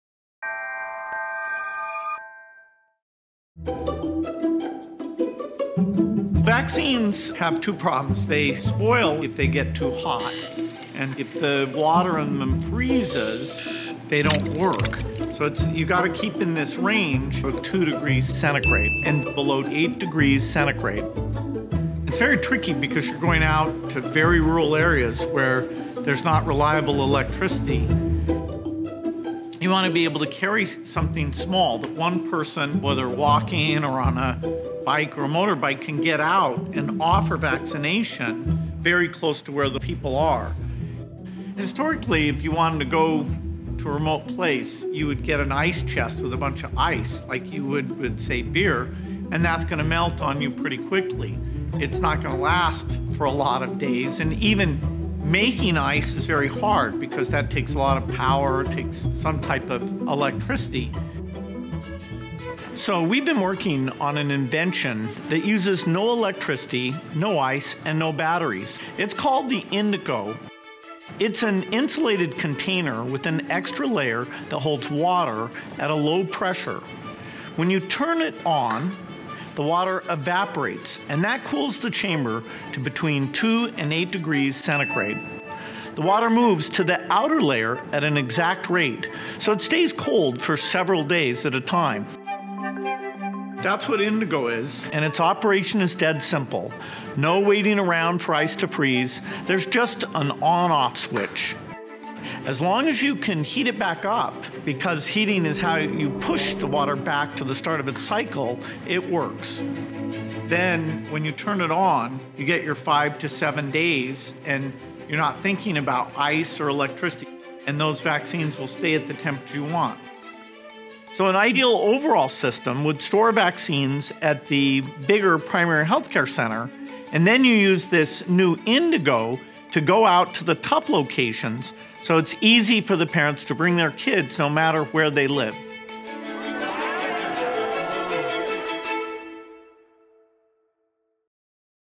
Indigo
Bill Gates introduces the Indigo, the remarkable invention that uses no electricity, no ice, and no batteries to store vaccines at the precise temperature required to remain effective.